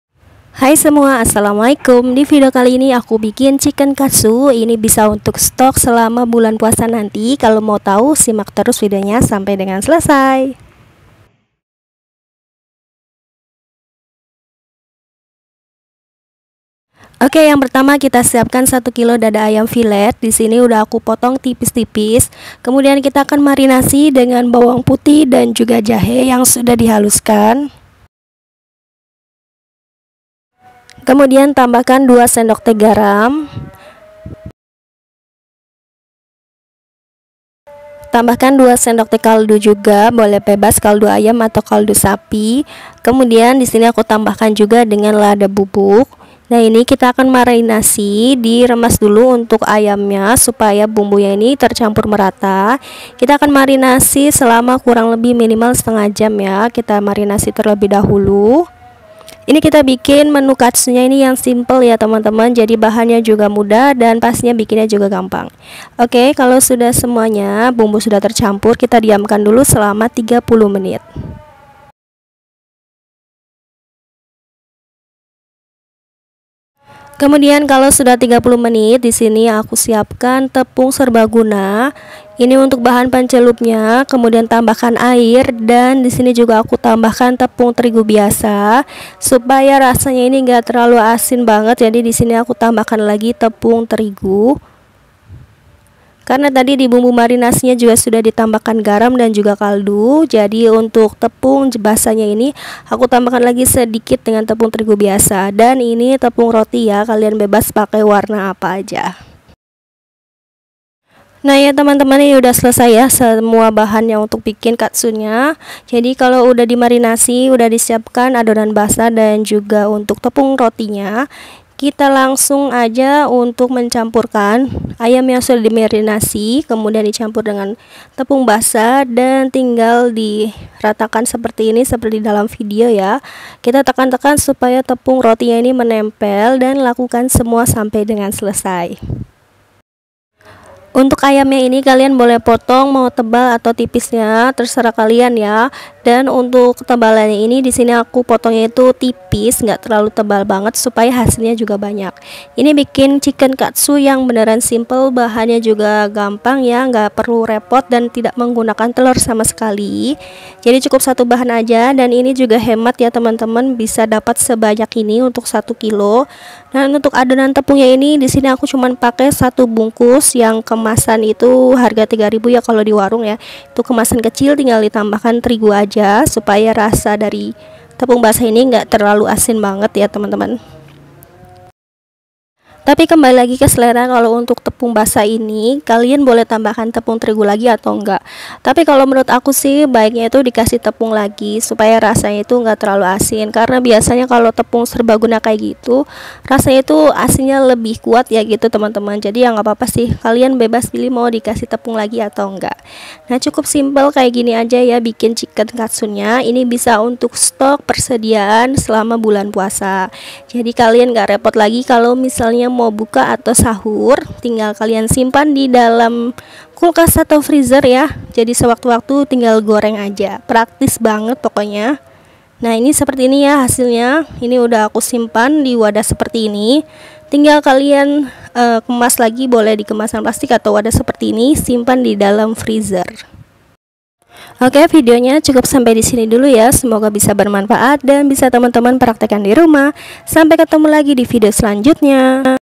CHICKEN KATSU EKONOMIS UNTUK JUALAN
assalamualaikum semua 😊✨

video kali ini aku mau share resep chicken katsu yang ekonomis dan wajib banget stok saat bulan puasa nanti 😊 simak videonya untuk cara membuatnya ya

🎀 Chicken Katsu 🎀

1kg dada ayam fillet potong tipis sesuai selera

bumbu marinasi
8 siung bawang putih
1/2 ruas jahe
2 sdt garam
2 sdt kaldu
1 sdt lada bubuk

bahan pencelup
1 bks tepung serbaguna
tepung roti secukupnya